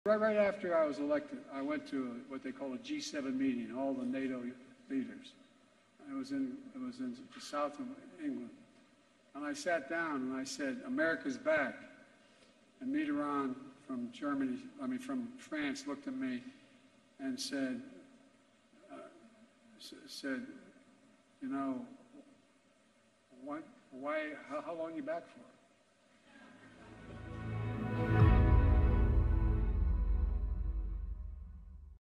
«Et Mitterand m’a dit…»: le lapsus gênant de Joe Biden en plein discours de campagne
Joe Biden a confondu Emmanuel Macron et François Mitterrand lors d'un rassemblement politique dimanche 4 février à Las Vegas. « Vous savez, juste après mon élection, je me suis rendu à ce qu'on appelle une réunion du G7, c'est-à-dire de tous les dirigeants de l'OTAN », a expliqué Biden au cours d'un discours, « C'était dans le sud de l'Angleterre. Je me suis assis et j'ai dit : L'Amérique est de retour ! Et Mitterrand, d'Allemagne - je veux dire, de France, m'a regardé et m'a demandé : pour combien de temps êtes-vous de retour ?»